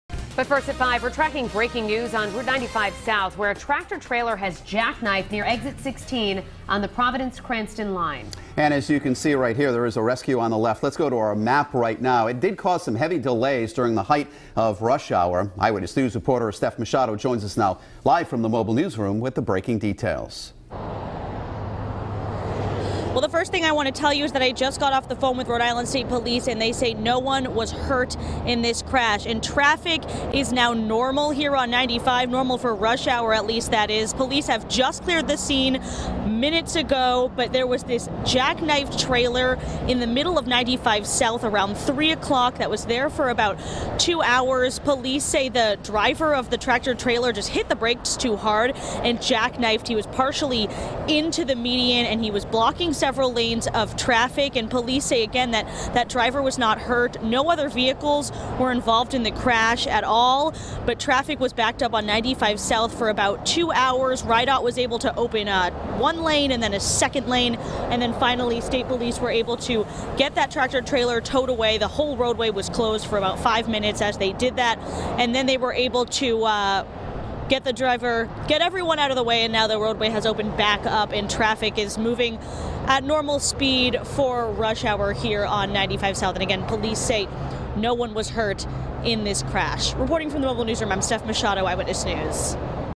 Tractor Trailer Crashes on I-95 SB
The crash blocked three lanes and tied up traffic for some time.